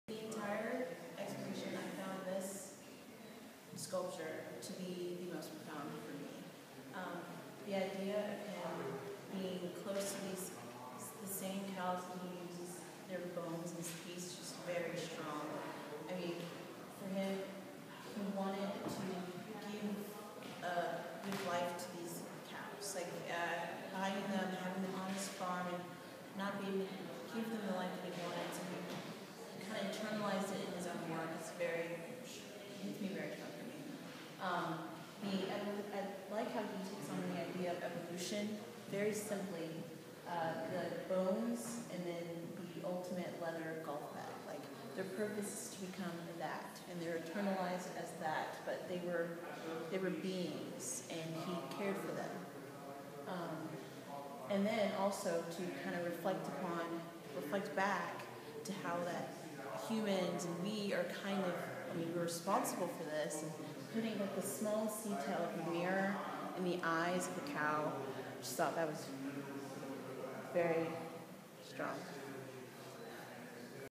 Thornton Dial and MLK 3
ArtsVibe Teen Council members respond to Thornton Dial's works of art in the exhibition "Hard Truths: The Art of Thornton Dial" at the High Museum of Art, emphasizing the influence of Dr. Martin Luther King, Jr on Dial's work and the world.